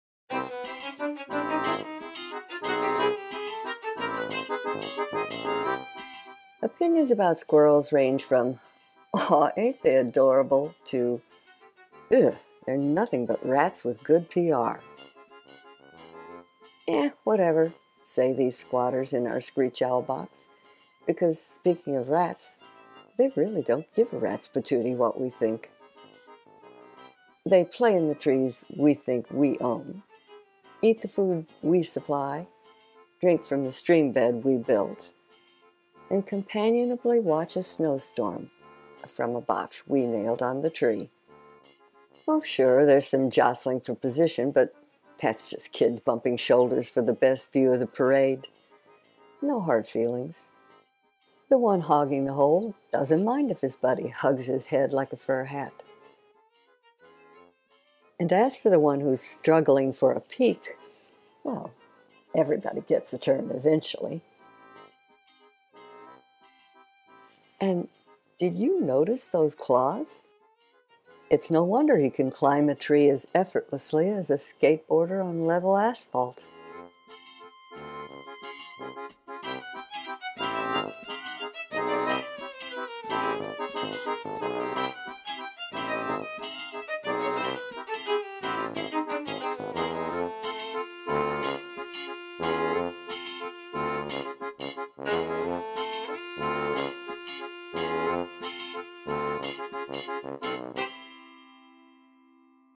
Squirrels Playful ... and Contented: NARRATED
Take a peek at these squirrels. They're sometimes playful, other times content to watch the world from their nest. PLEASE CLICK 'LIKE' IF YOU APPRECIATE THE MOVIE.